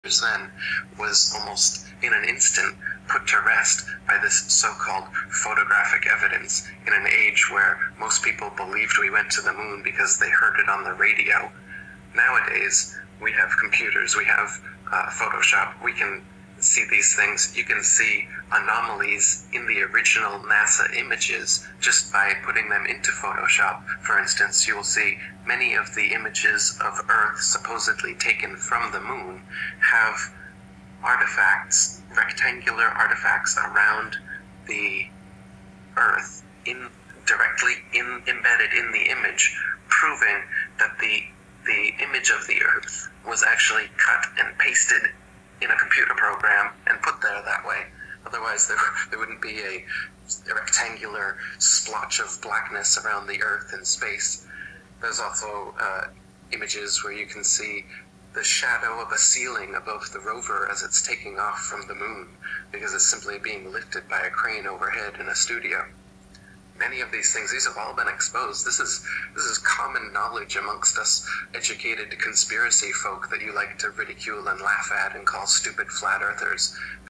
Official NASA photos in Photoshop reveal copy and paste
Download official NASA photos from their website and open them up on Photoshop and adjust the brightness and contrast to see obvious copy and past ( boxes ) anomalies. Eric Dubay explains